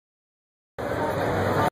Short Video of Crook & Chase at CMA Festival in Nashville Tennessee. Fan fair Music City Center
Video of Crook & Chase at CMA fest 06/08/2013    Philfeedback Awesome Great fun Cool Have Nice Day Tour Tourism Travel adventure culture destination Nature Wildlife Neat events Event.